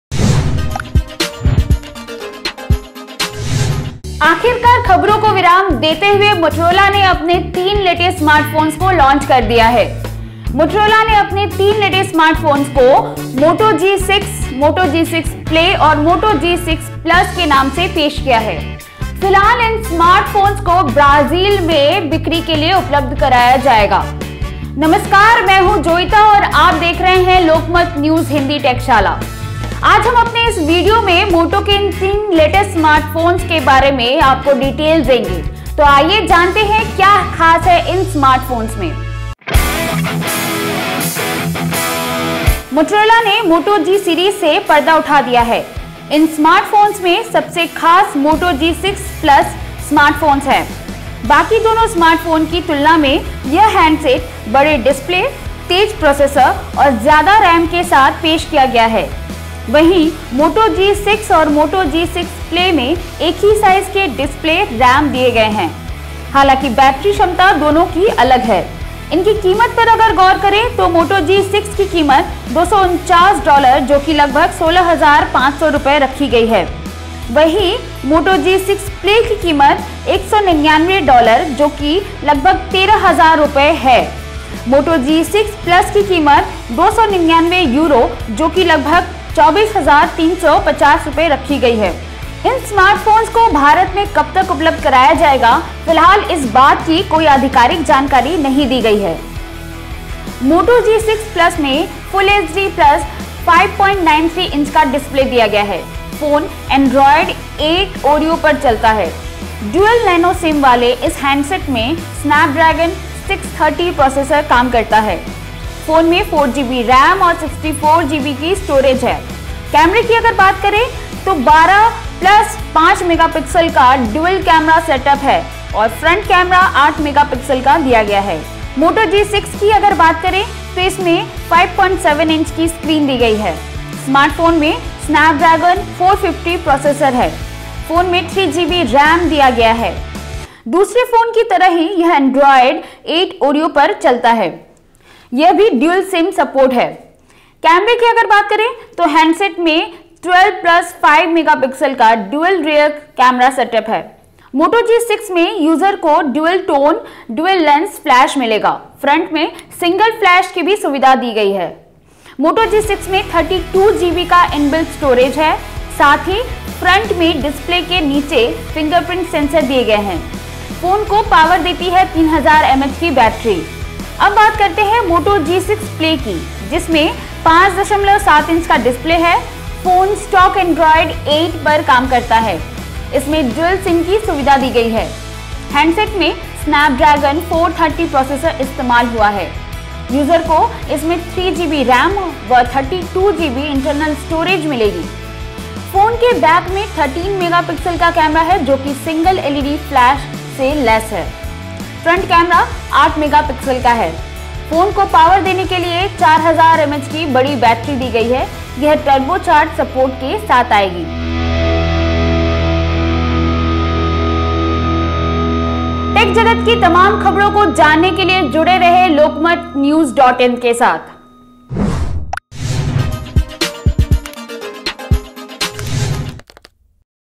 Motorola Moto G6 | G6 Plus | G6 Play | Launched | Large screens | Big Batteries
The Moto G6, Moto G6 Plus, and Moto G6 Play smartphones have finally become official launched. The new Moto G6 Plus sits at the top of the series with a marginally bigger display, faster processor, and more RAM than its two siblings. On the other hand, Moto G6 and Moto G6 Play have the same display size and RAM, but vary in terms of battery capacity, camera configuration, and the SoC.